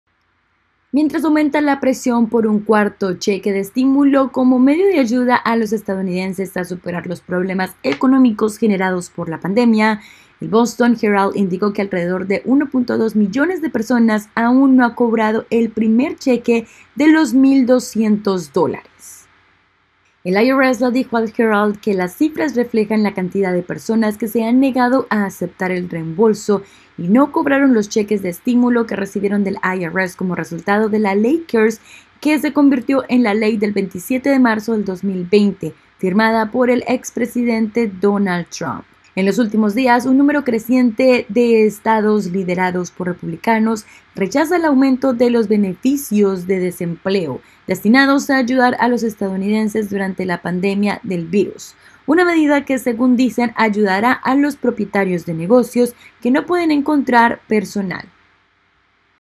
Alrededor de 1,2 millones de personas no han cobrado su primer cheque de estímulo | Noticias
Mientras aumenta la presión por un cuarto cheque de estímulo como medida para ayudar a los estadounidenses a superar los problemas económicos generados por la pandemia, el Boston Herald indicó que alrededor de 1,2 millones de personas aún no han cobrado su primer cheque de 1,200 dólares.